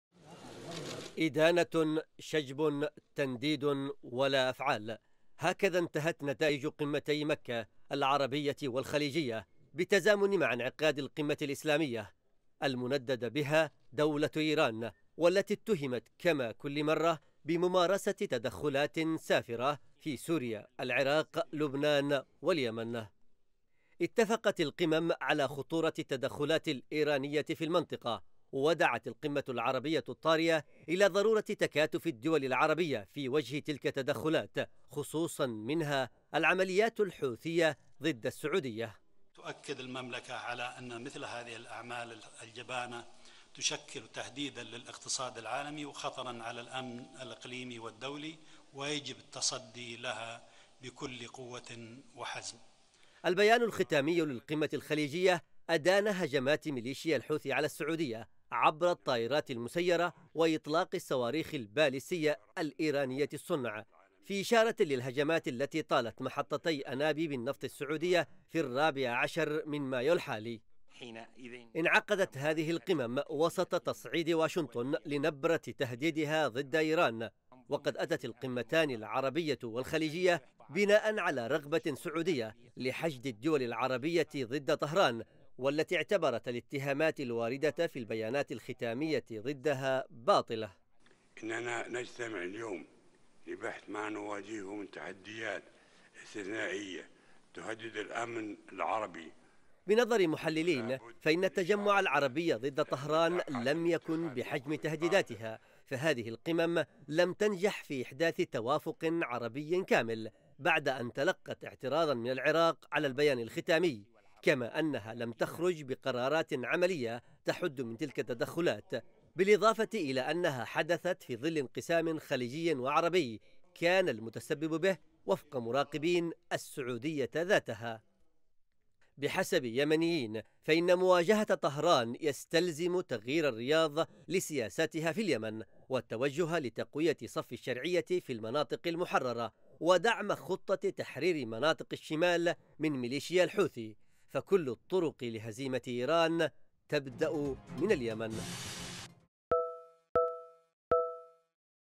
مجلس التعاون الخليجي يدين التدخلات الإيرانية وهجمات الحوثي على مصالح سعودية|تقرير: محمد اللطيفي
أدان مجلس التعاون الخليجي التدخلاتِ الإيرانيةَ في المنطقة، والهجماتِ التي نفذتها مليشيا الحوثي على محطتي ضخِ نفطٍ بالسعودية، وتعرضَ أربعِ سفنٍ تجاريةٍ للتخريبِ قربَ المياهِ الإقليميةِ الإماراتية.
وعبر قادةُ مجلس التعاون عن إدانتهم للهجمات التي قامت بها الميلشيا الحوثية باستخدام طائرات مسيرة مفخخة واستمرارها في إطلاق الصواريخ البالستية. ..